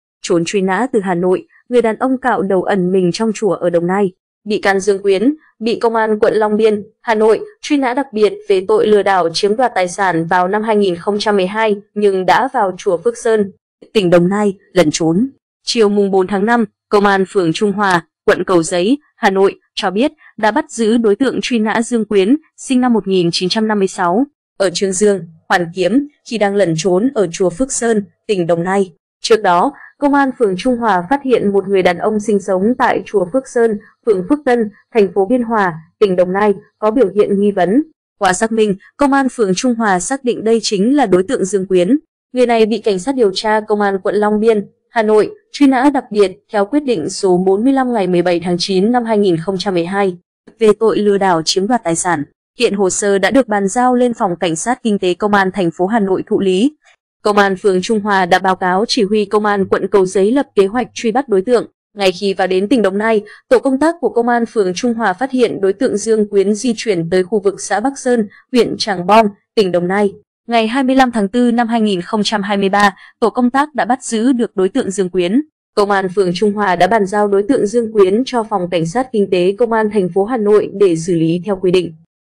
Trốn truy nã từ Hà Nội, người đàn ông cạo đầu ẩn mình trong chùa ở Đồng Nai
Bị can Dương Quyến bị Công an quận Long Biên (Hà Nội) truy nã đặc biệt về tội lừa đảo chiếm đoạt tài sản vào năm 2012 nhưng đã vào chùa Phước Sơn (tỉnh Đồng Nai) lẩn trốn.Ngay khi vào đến tỉnh Đồng Nai, Tổ công tác của Công an phường Trung Hòa phát hiện đối tượng Dương Quyến di chuyển tới khu vực xã Bắc Sơn, huyện Trảng Bom, tỉnh Đồng Nai.
Bị can Dương Quyến bị cơ quan công an bắt.
Chiều 4/5, Công an phường Trung Hòa, quận Cầu Giấy (Hà Nội) cho biết, đã bắt giữ đối tượng truy nã Dương Quyến (SN 1956, ở Chương Dương, Hoàn Kiếm) khi đang lẩn trốn ở chùa Phước Sơn, tỉnh Đồng Nai.
Qua xác minh, Công an phường Trung Hòa xác định đây chính là đối tượng Dương Quyến. Người này bị CSĐT Công an quận Long Biên (Hà Nội) truy nã đặc biệt theo quyết định số 45 ngày 17/9/2012 về tội lừa đảo chiếm đoạt tài sản. Hiện hồ sơ đã được bàn giao lên Phòng Cảnh sát kinh tế Công an TP Hà Nội thụ lý.
Công an phường Trung Hòa đã báo cáo Chỉ huy Công an quận Cầu Giấy lập kế hoạch truy bắt đối tượng.
Ngày 25/4/2023, Tổ công tác đã bắt giữ được đối tượng Dương Quyến.
Trước đó, Công an phường Trung Hòa phát hiện một người đàn ông sinh sống tại chùa Phước Sơn, phường Phước Tân, TP Biên Hòa (tỉnh Đồng Nai) có biểu hiện nghi vấn.
Công an phường Trung Hòa đã bàn giao đối tượng Dương Quyến cho Phòng Cảnh sát kinh tế CATP Hà Nội để xử lý theo quy định.